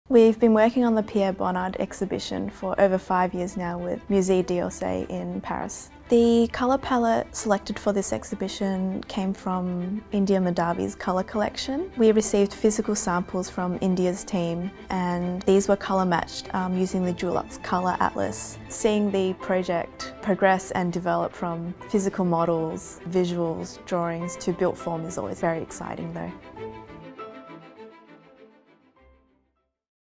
Pierre Bonnard Exhibition | Dulux
Melbourne Winter Masterpieces® 2023: Pierre Bonnard: Designed by India Mahdavi Experience the mesmerising colours of Dulux Smooth Beech and Dulux Befitting gracing the walls of the NGV as the perfect backdrop to the artwork of Pierre Bonnard. Crafted by India Mahdavi, the captivating designs reflect the timeless elegance of Bonnard's repertoire. 🎨 Colours featured: DuluxSmoothBeech , DuluxBefitting . 🖌️ Paint featured: DuluxWashandWear 🛋 | India Mahdavi | NGV Melbourne 🖼 | Pierre Bonnard